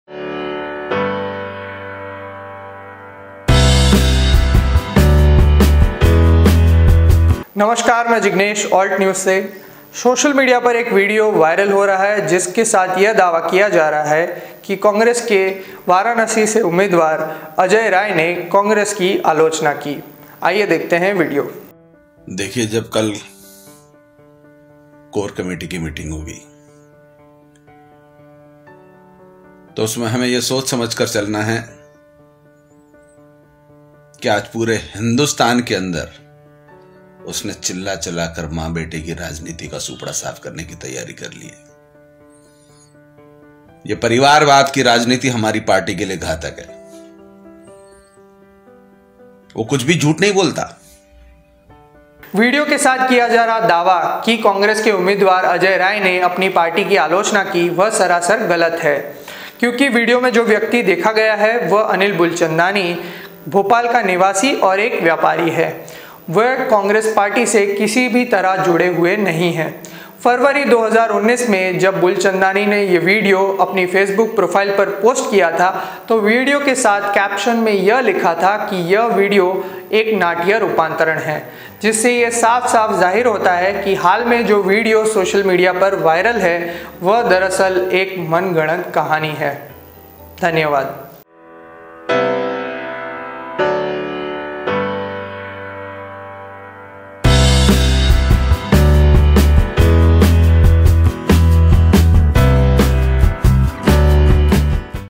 क्या कॉंग्रेस नेता अजय राय ने गांधी परिवार की आलोचना की?
सोशल मीडिया पर वायरल हो रहे वीडियो में गांधी परिवार की आलोचना कर रहे व्यक्ति को कॉंग्रेस नेता अजय राय बताया जा रहा है। जबकि वीडियो में दिख रहे व्यक्ति कोई कॉंग्रेस नेता नहीं बल्कि एक भाजपा समर्थक हैं।

Social media par viral video me congress ki alochna kar rahe vyakti ko congress neta Ajay rai bataya jaa raha hai. lekin video me dikh rhe vyakti koi congress neta nahi balki ek bjp samarthak hain.